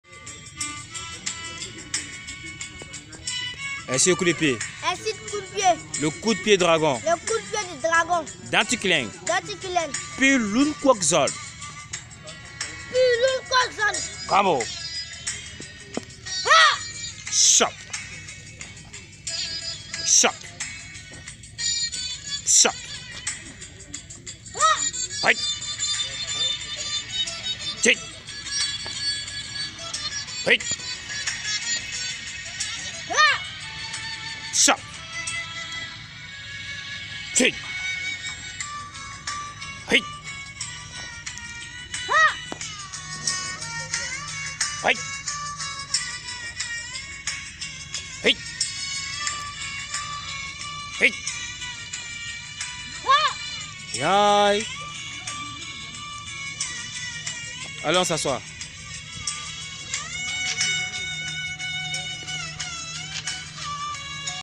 Planshe kato à l'école martiale de l'aigle
Le coup pieds du dragon de l'école martiale de l'aigle